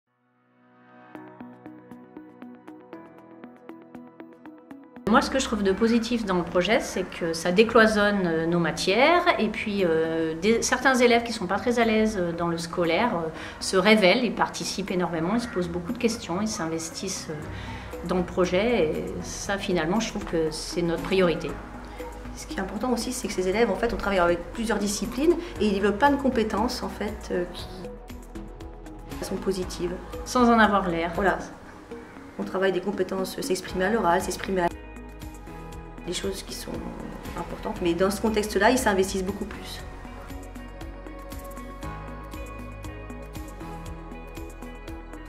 Regards Croisés 2023-2024 | Témoignage de deux enseignantes de Jongkind ( La Côte Saint-André).
Cette vidéo est un témoignage de deux des professeurs accompagnants (es) du projet sur la thématique de Nature/Culture avec des élèves de 5ème qu'elles accompagnent. Après seulement trois mois avec le projet, elles nous font part de leur avis sur Regards Croisés.